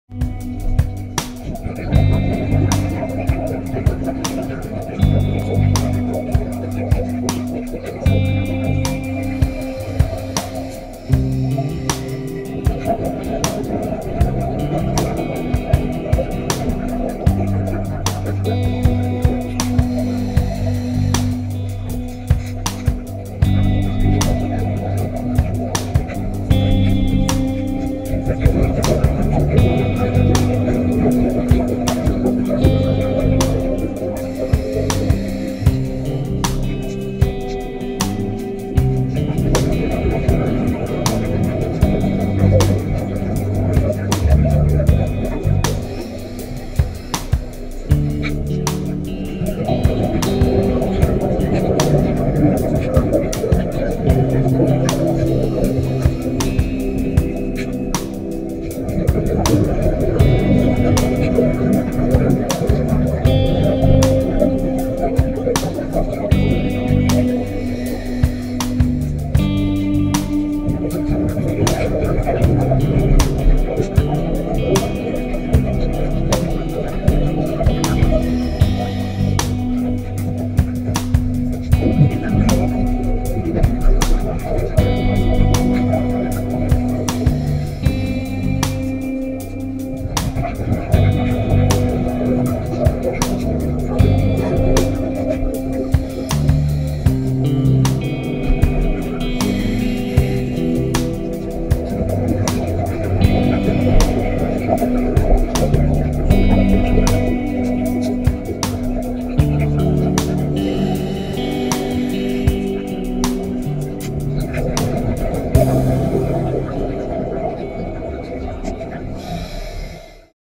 Jennifer SCUBA Diving at the Blue Heron Bridge on 8-13-2020
Jennifer diving at Blue Heron Bridge